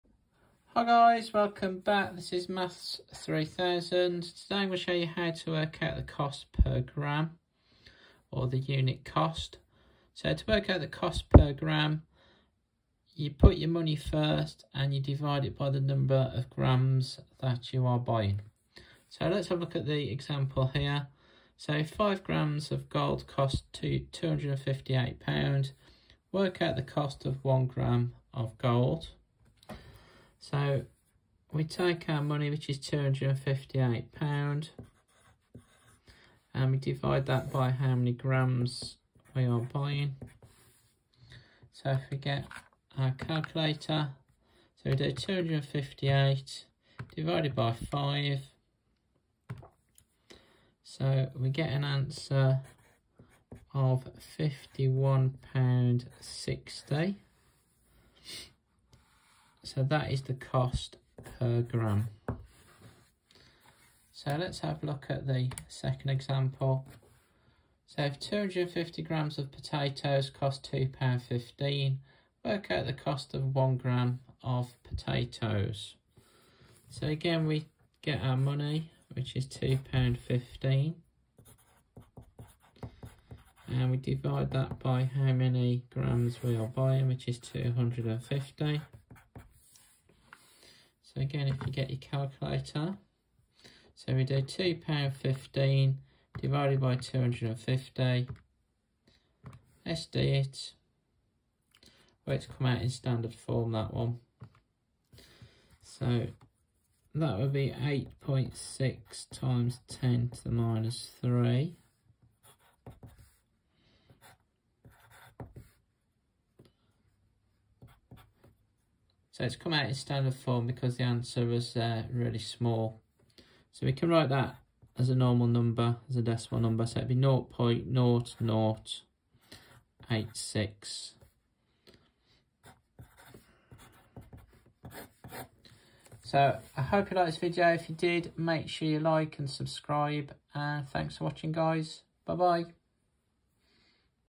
How To Work Out The Cost Per Gram (Price of 1g - Unit Price)
In this video you will learn how to work out the cost per gram. To do this divide the money by how many grams you are buying.

So in the first example, 5g of gold cost £258 and you are asked to work out the cost per gram. To do this divide £258 by 5 to give £51.60. So £51.60 is the cost per 1g of gold.

In the second example, 215g of potatoes cost £2.15 and you are asked to find the cost per gram. To do this divide £2.15 by 215 to give 0.0086. So for 1g of potatoes is Costa £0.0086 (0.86 pence).